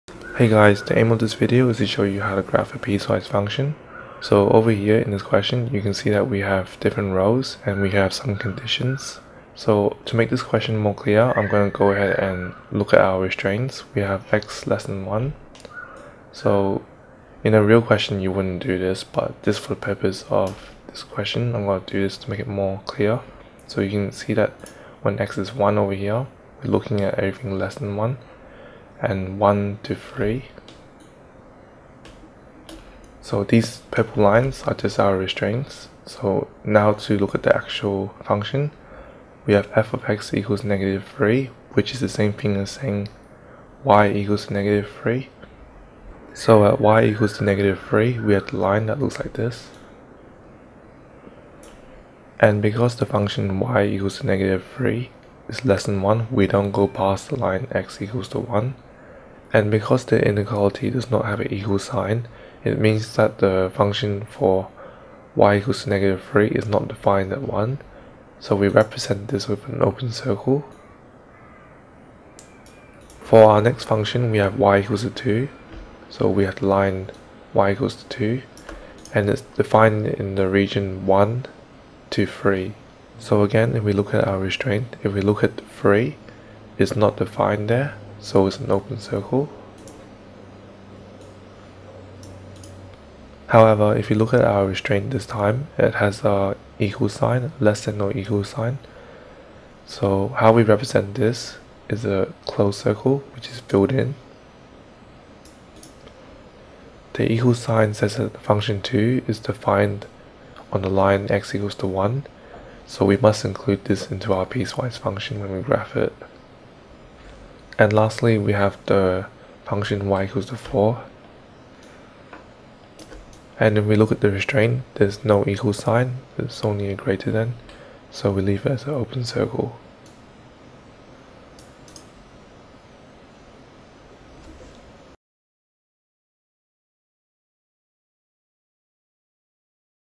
How to graph piecewise functions open or closed circles
Learn how to graph piecewise functions from a worked example demonstrating if you should use open or closed circles